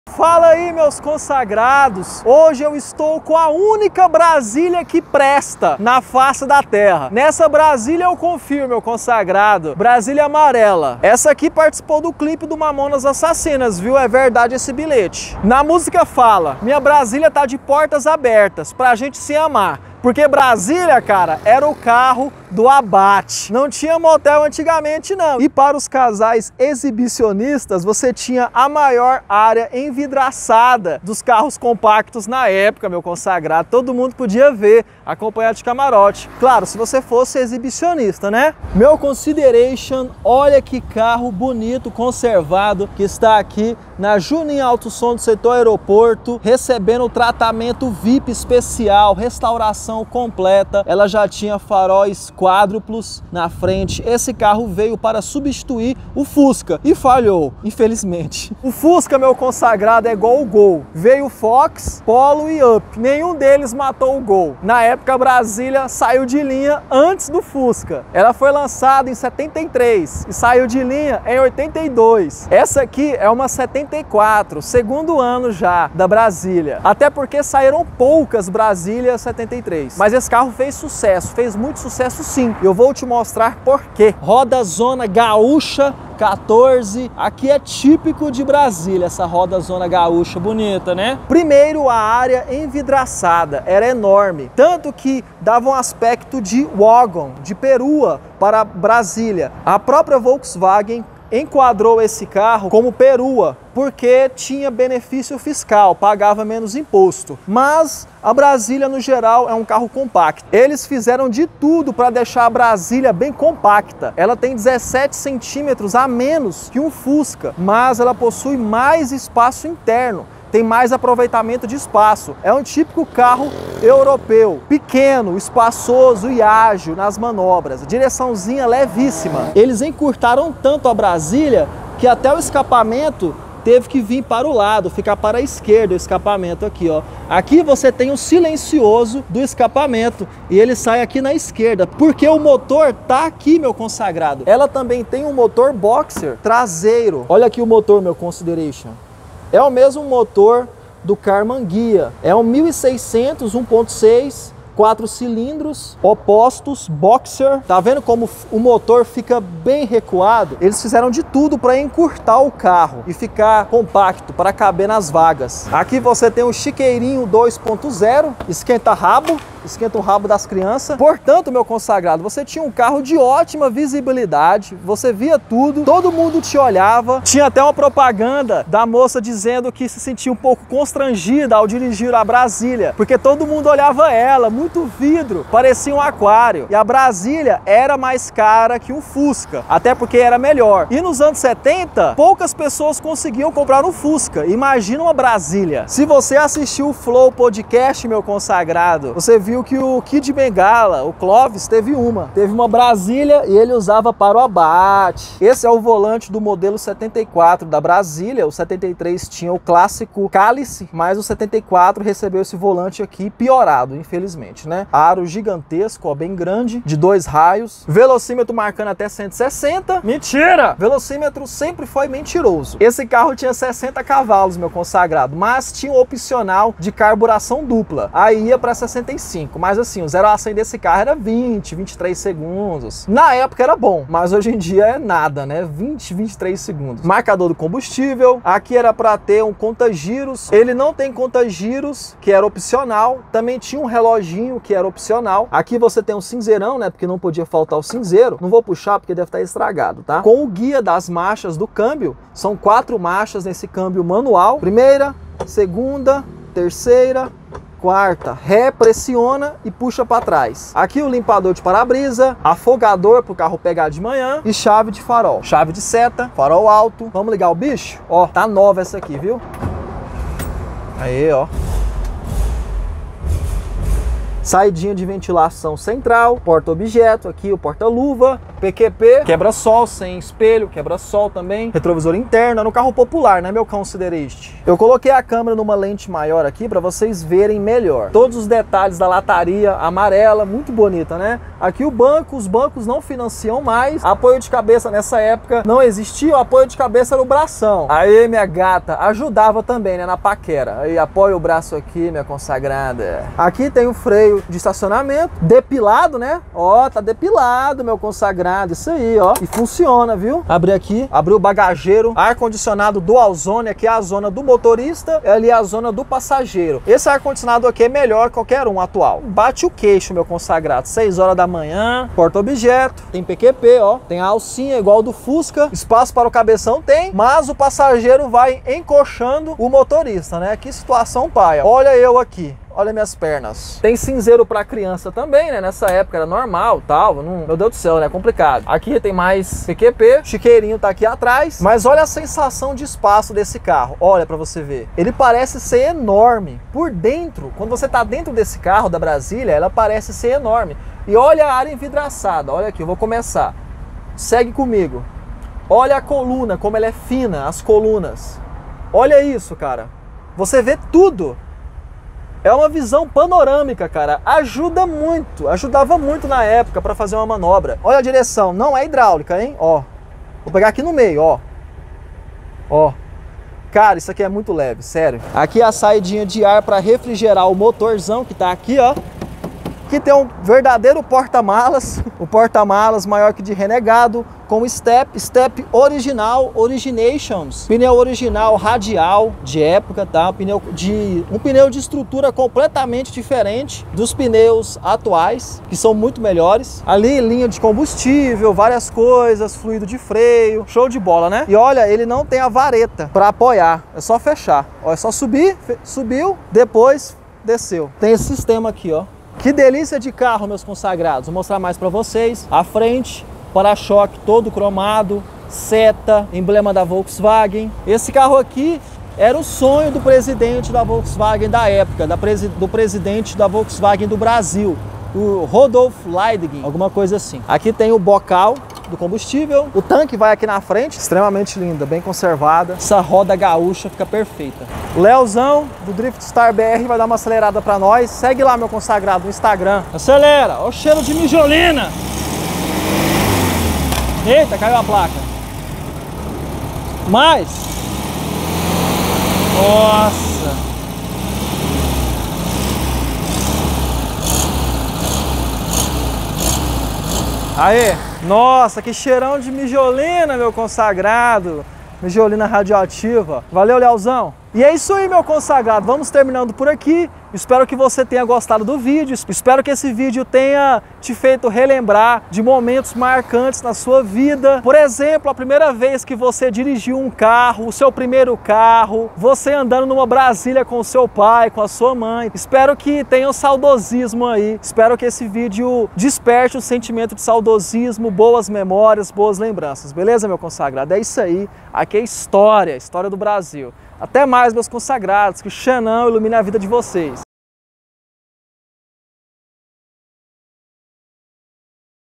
Volkswagen Brasília 1974, a verdadeira Brasília amarela. Relíquia, carro de colecionador.
Encontrei a verdadeira Brasília amarela! Essa é 1974, do segundo ano de produção. Ela foi feita para substituir o Fuscão mas acabou saindo de linha antes, em 1982. O carro impressionou na época pela excelente visibilidade, por causa da sua área envidraçada enorme. Era um carro de fácil estacionar, compacto e confortável!

Consulta completa do histórico do veículo com desconto O XENÃO RECOMENDA!